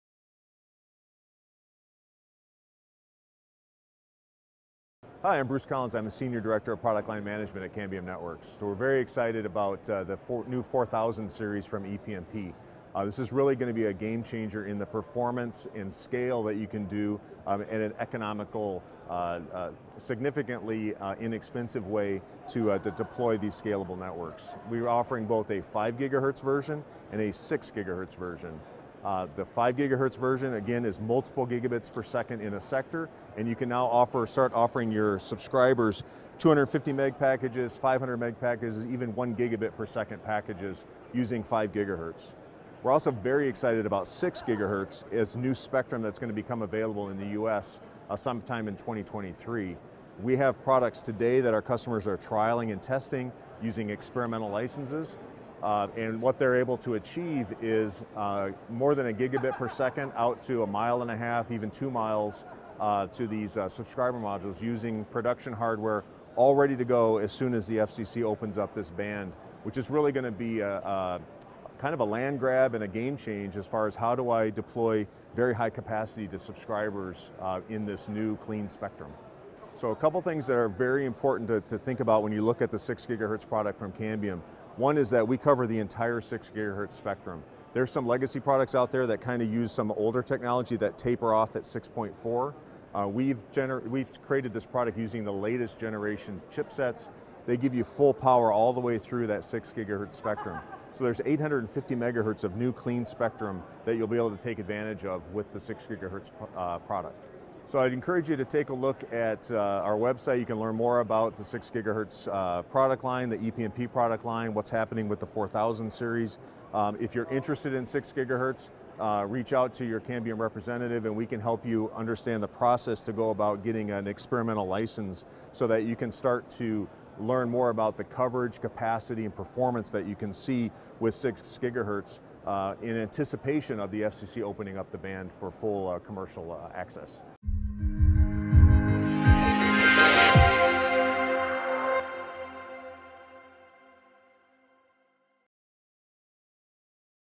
ePMP 4600 6 GHz Fixed Wireless Introduction
Make the most of 6 GHz with the ePMP 4600 fixed wireless solution that delivers:
• High-performance, scalable and reliable access points for fixed wireless broadband
• ePMP 4600 features MU-MIMO for up to 4 Gbps in 6 GHz
• Low TCO with three-year hardware warranty
• Interoperable with all Force 4600 radios